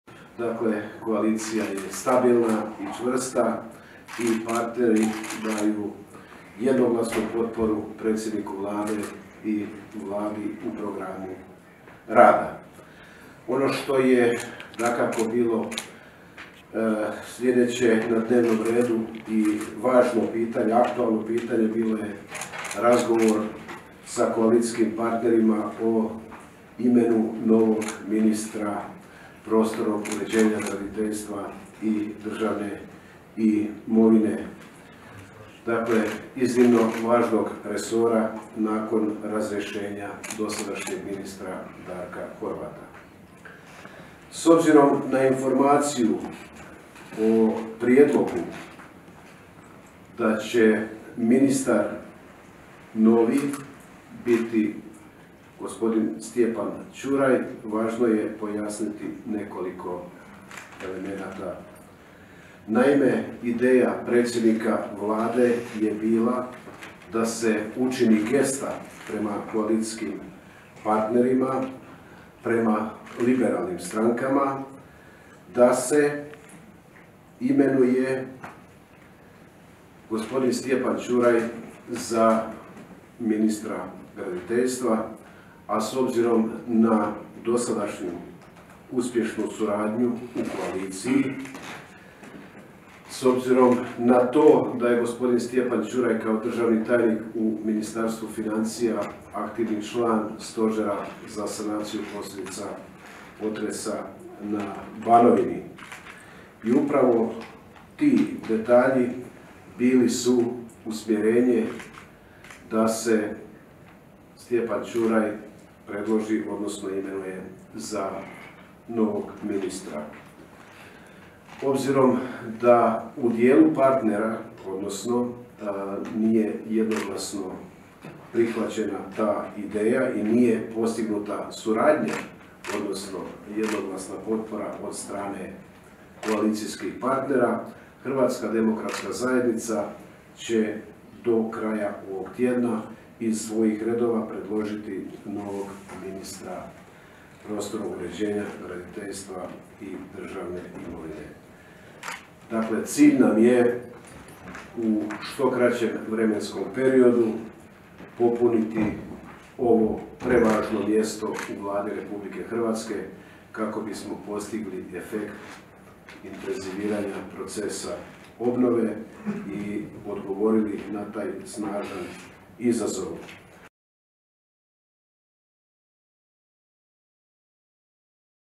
Tomo Medved: Novi ministar graditeljstva bit će iz redova HDZ-a
U Banskim dvorima, nakon kolacijskog sastanka partnera u Vladi, izjavu za medije dao je potpredsjednik Vlade i ministar branitelja Tomo Medved.